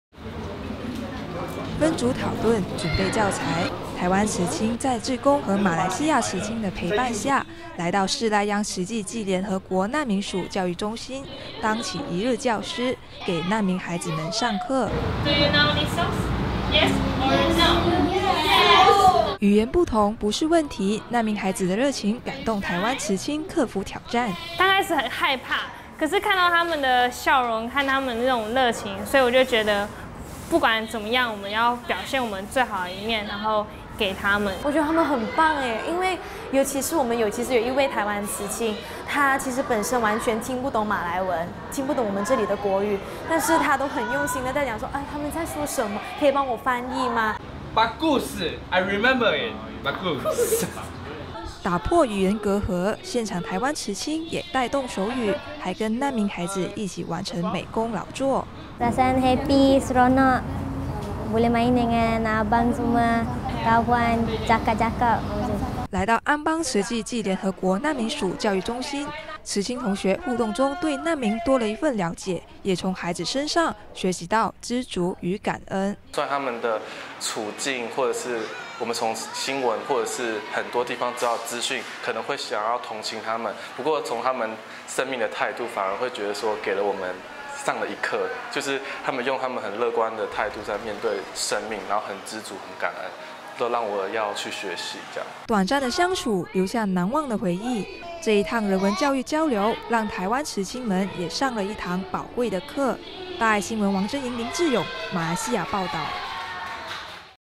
慈青訪難民學校 見苦知福習感恩[HD]
把握暑假為心靈充電，台灣有一群慈青前往馬來西亞吉隆坡，進行人文教育交流，走訪了當地兩所慈濟暨聯合國難民署教育中心，年輕活力的慈青同學教導難民孩子音樂和美術課。實際付出讓慈青們見苦知福，也學習知足與感恩。

分組討論，準備教材，台灣慈青在志工和馬來西亞慈青的陪伴下，來到士拉央慈濟暨聯合國難民署教育中心，當起一日教師，給難民孩子們上課。

你們知道這首歌嗎？

語言不同，不是問題。難民孩子的熱情，感動台灣慈青克服挑戰。

台灣慈青 林佳儀 Lin, Chia-Yi
剛開始很害怕
可是看到他們的笑容和他們的那種熱情
所以我就覺得不管怎麼樣
我們要表現我們最好的一面
然後給他們這樣

大馬慈青 林雯靖Lim Men Jing
我覺得他們很棒
因為尤其是其實我們有一位台灣慈青
他其實本身完全聽不懂馬來文
聽不懂我們這裡的國語
但是他們都很用心地講說
他們在說什麼 可以幫我翻譯嗎

台灣慈青:bagus(很棒） , I remember it (我記得）

打破語言隔閡，現場台灣慈青也帶動手語，還跟難民孩子一起完成美工勞作。

難民學生  莎西娜Shahina
覺得很開心很好玩perasaan happy seronok
可以和哥哥他們玩boleh main dengan abang semua
交朋友 和聊天kawan， cakap cakap

 安邦難民學校畫面
來到安邦慈濟暨聯合國難民署教育中心。慈青同學互動中對難民多了一份了解，也從孩子身上，學習到知足與感恩。

台灣慈青  王威勝
雖然他們的處境
我們從新聞或者是很多地方得到資訊
可能會想要同情他們
不過從他們生命的態度反而會給我們上了一課
他們用他們很樂觀的態度在面對生命
然後很知足  很感恩
讓我要去學習這樣

短暫的相處，留下難忘的回憶。這一趟人文教育交流，讓台灣慈青們也上了一堂寶貴的課。

大愛新聞 王楨瑩 林志勇 馬來西亞 報導